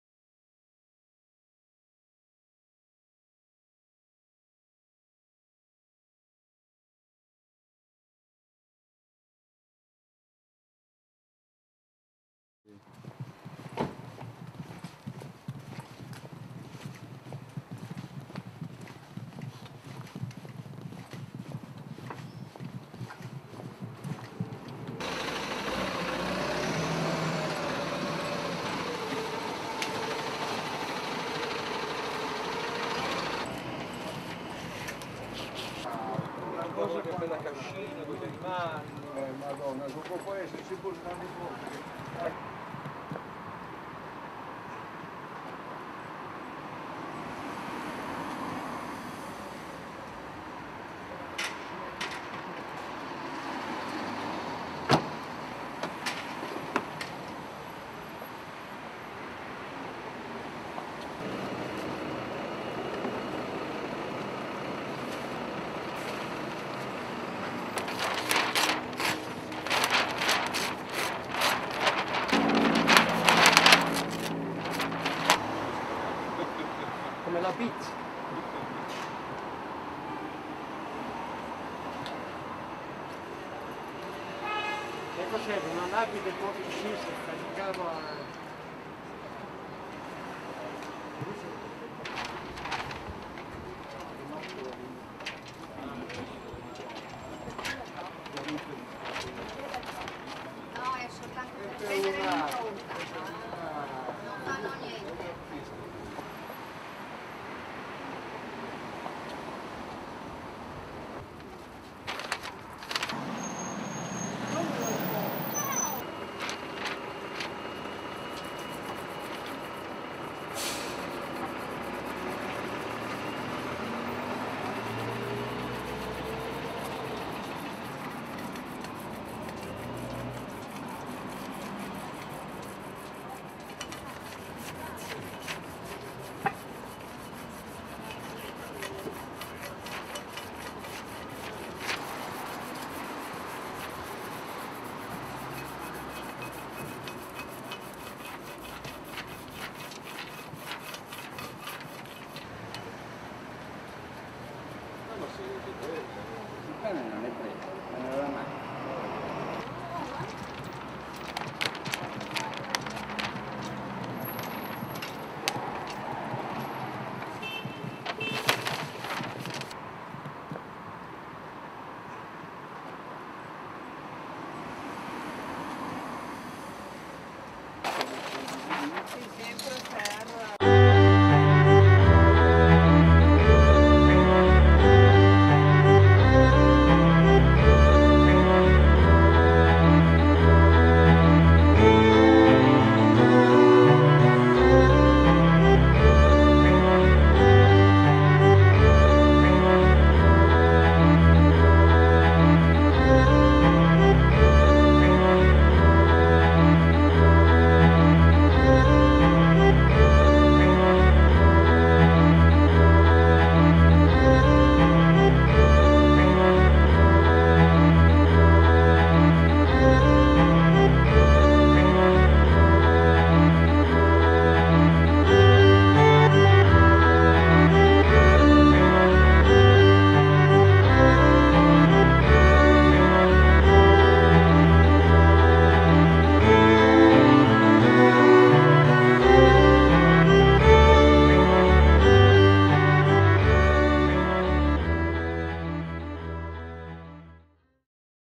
ANPI Barona Milano - farememoria
Una "nostra" lapide... Via Biella - Milano. Intervento in Frottage di Giovanni Rubino. FAREMEMORIA..